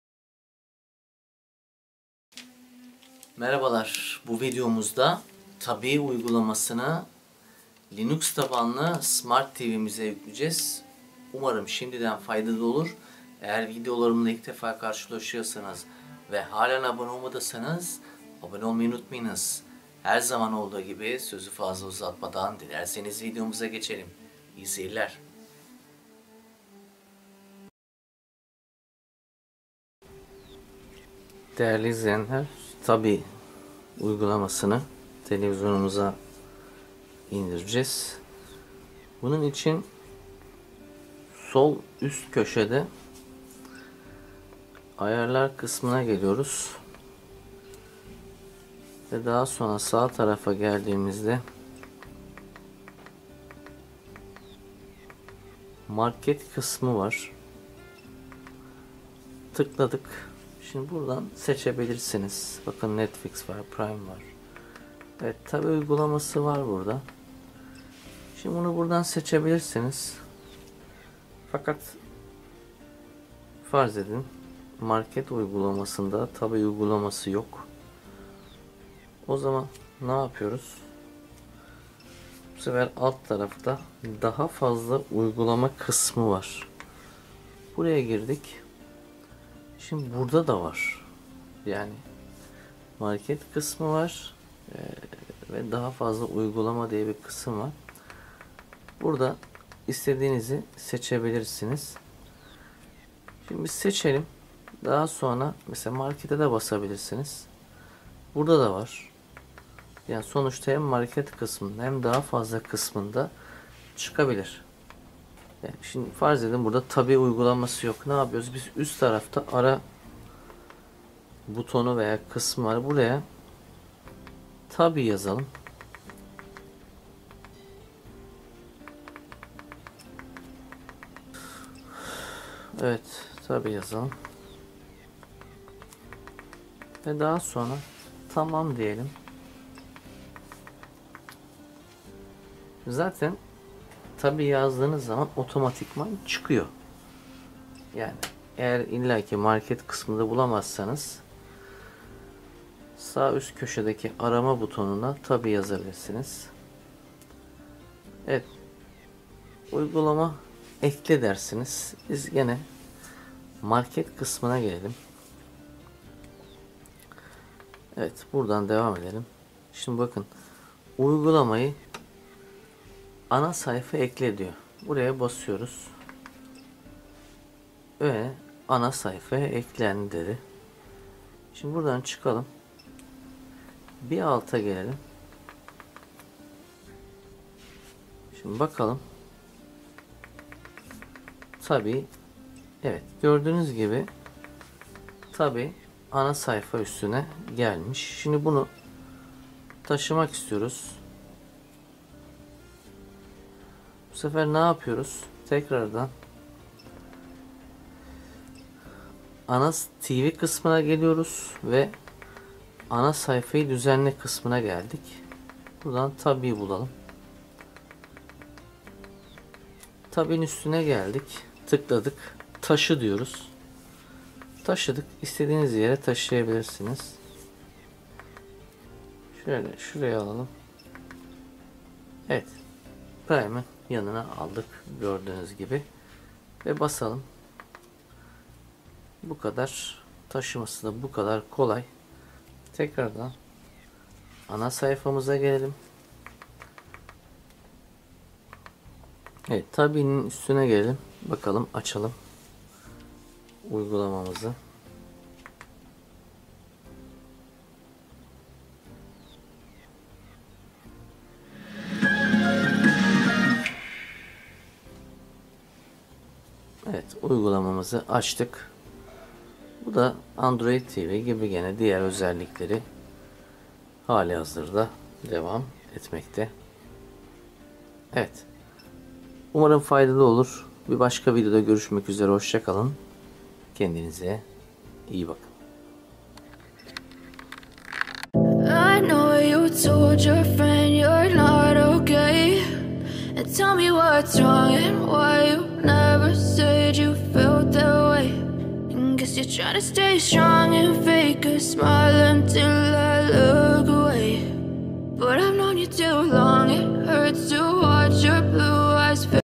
TABİİ SMART TV LİNUX SİSTEME KURMAK
LİNUX İŞLETİM SİSTEMLİ VESTEL,SEG,REGAL MARKA TV LERDE VE DİGER LİNUX SİSTEMLİ TVLERDE NASIL KURULUR BU VİDEODA.
00:00 TANITIM BİLGİLENDİRME
00:26 TABİİ KURMAK
03:00 TABİİ TAŞIMA
04:47 BİTİŞ EKRANI